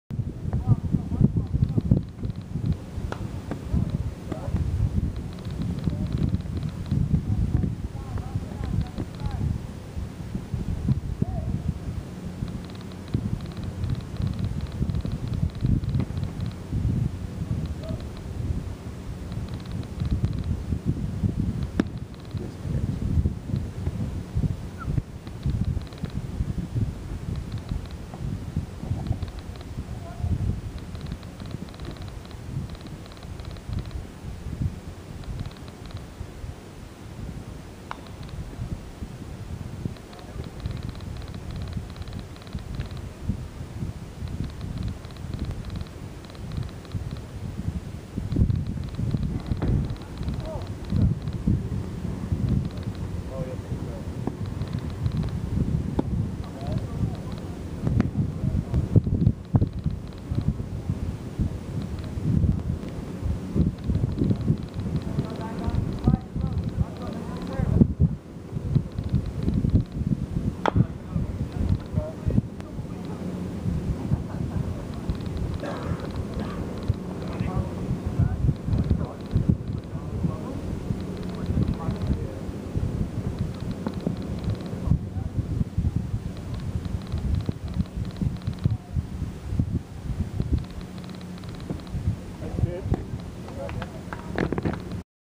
James Pitching/ Mixing pitches-strikeout swinging with curve ball
vs. Menifee Mustangs
10-20-13